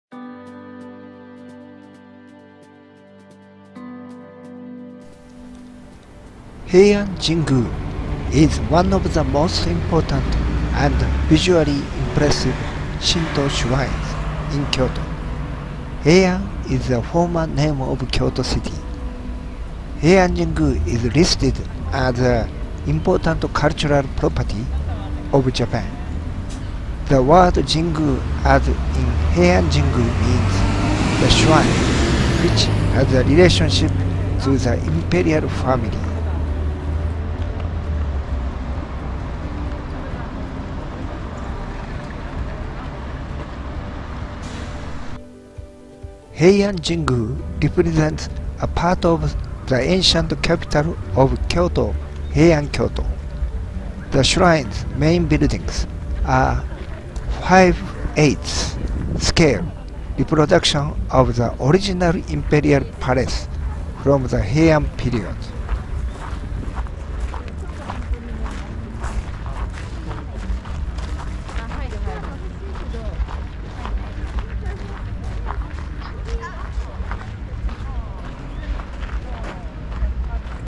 Heian jingu overview2
Heian-jingu is one of the most important and visually impressive Shinto shrines in Kyoto.